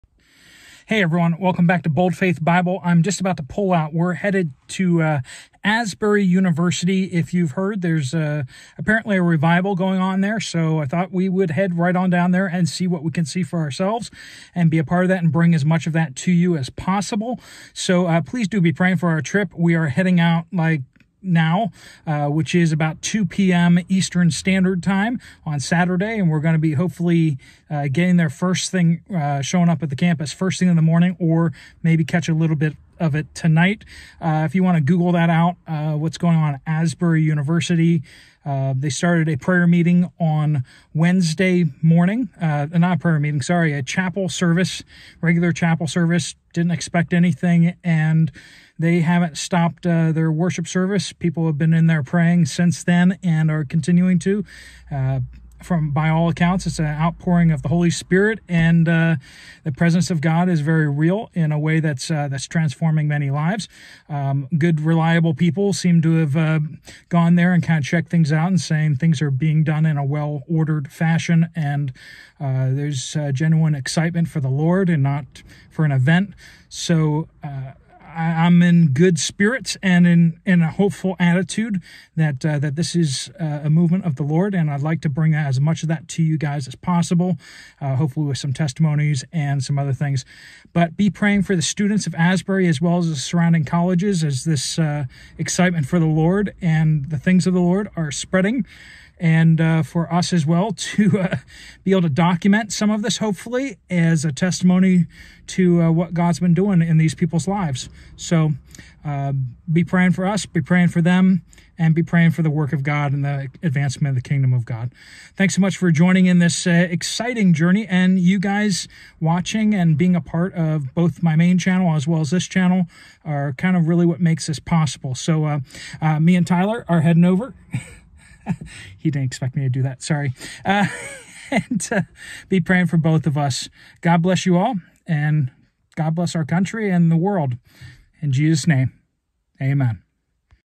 Revival Road Trip To Asbury University 2023
Reports are now widespread of a special revival at AsburyUniversity in Lexington Kentucky.  Please pray for safety in travel and a special move of God amongst our channel viewers and many around the US and around the world.